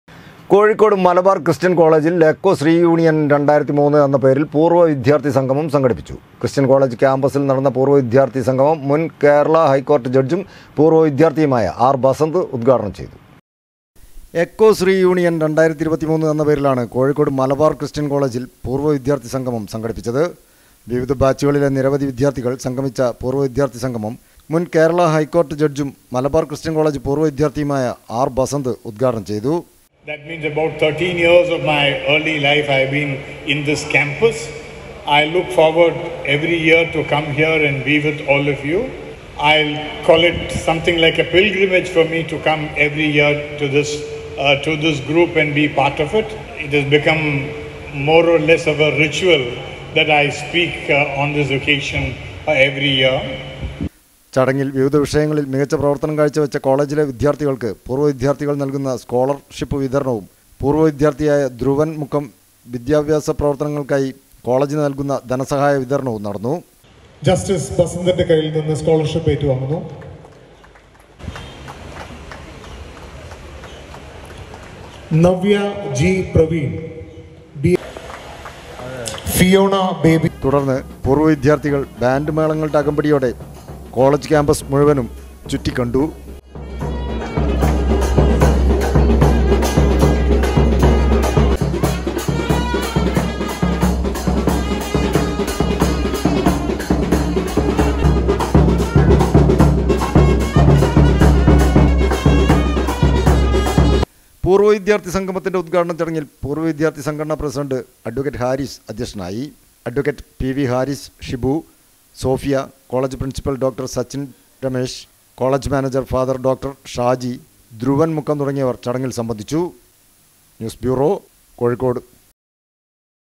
കോഴിക്കോട് മലബാർ ക്രിസ്ത്യൻ കോളേജിൽ എക്കോസ് റീയൂണിയൻ 2003 എന്ന പേരിൽ പൂർവ്വ വിദ്യാർത്ഥി സംഗമം
കോഴിക്കോട് മലബാർ ക്രിസ്ത്യൻ കോളേജിൽ എക്കോസ് റീയൂണിയൻ 2003 എന്ന പേരിൽ പൂർവ്വ വിദ്യാർത്ഥി സംഗമം സംഘടിപ്പിച്ചു